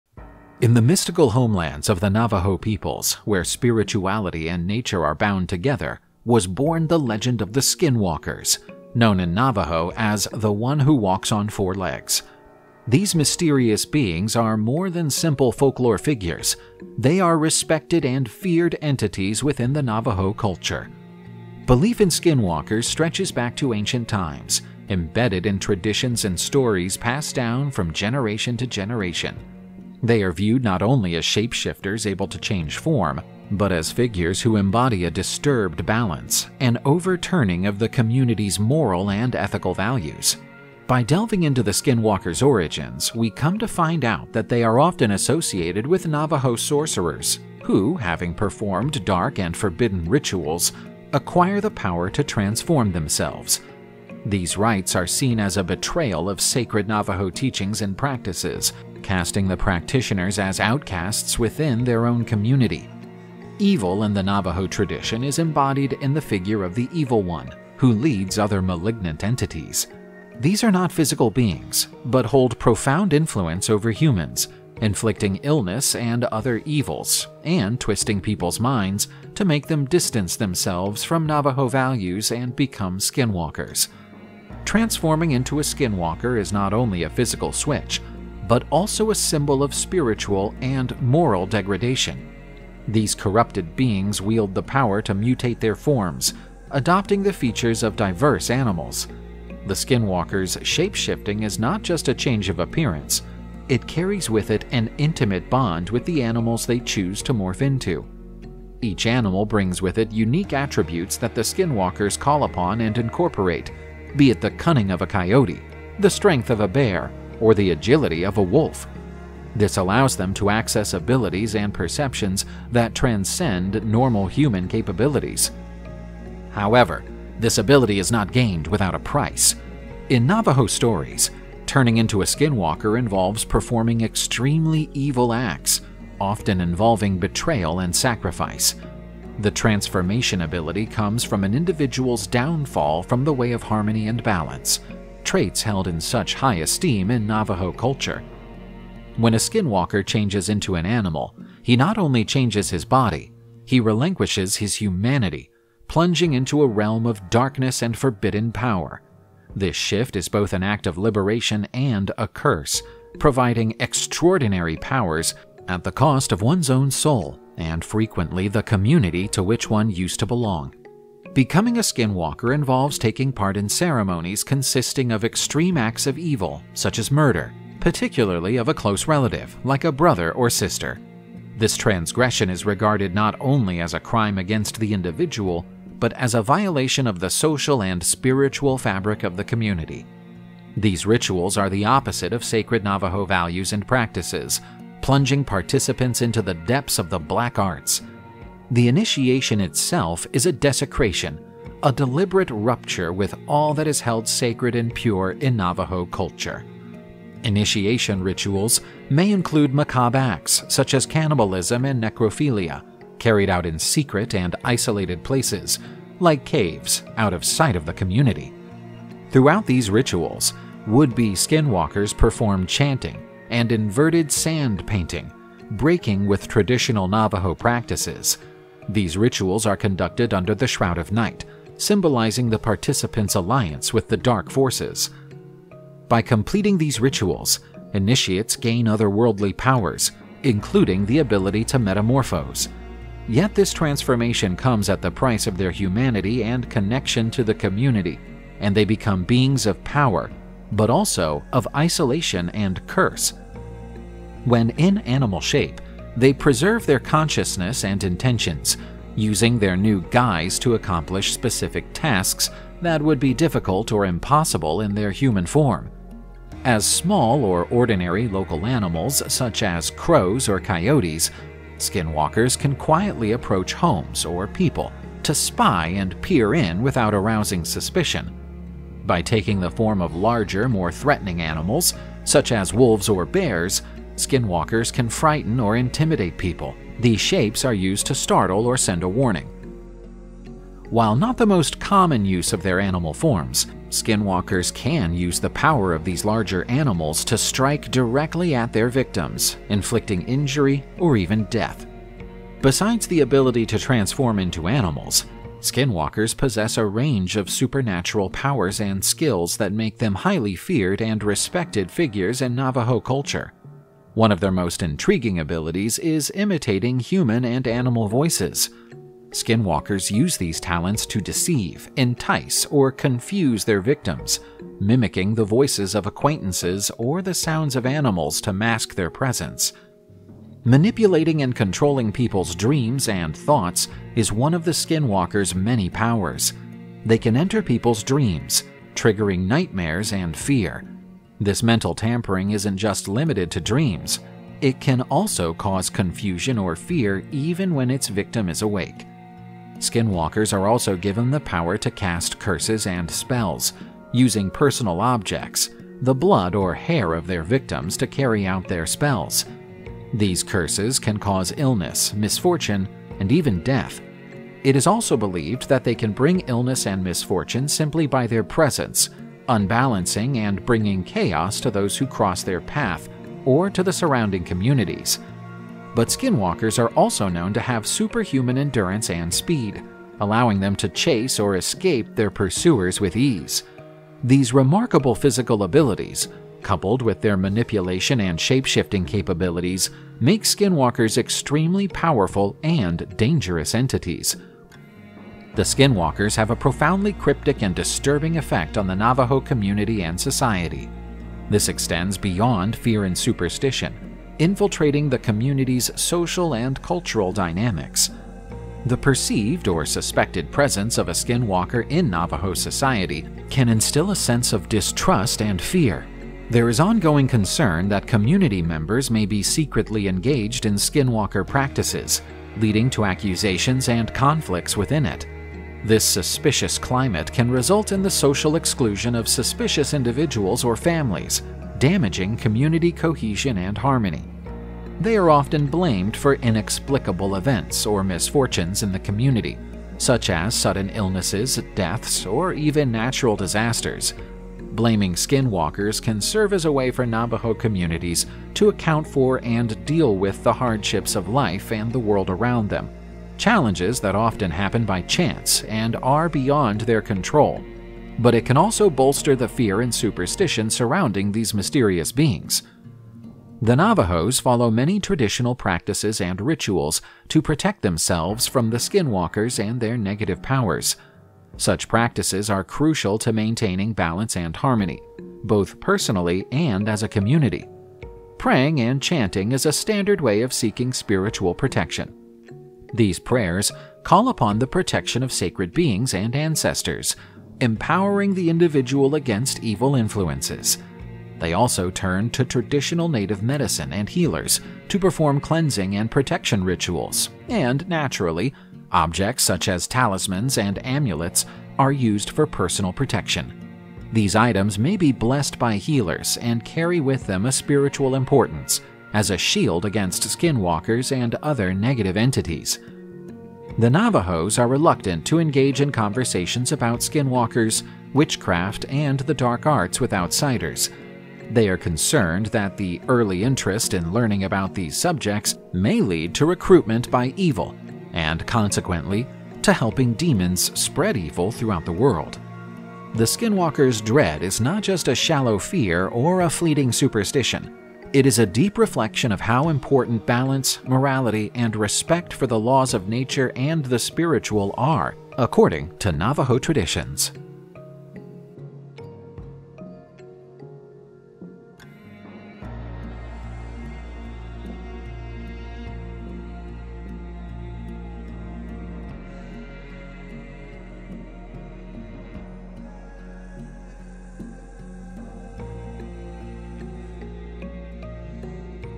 Skinwalker – The Terrifying Evil Entities of Navajo Culture
Skinwalkers are legends from Navajo folklore, which means "he who walks on four legs." According to the legend, they are witches who use animal skins to transform and gain powers. They use these powers to frighten or control others, shrouded in mystery and fear, because talking about them is taboo, believed to attract their malevolent attention.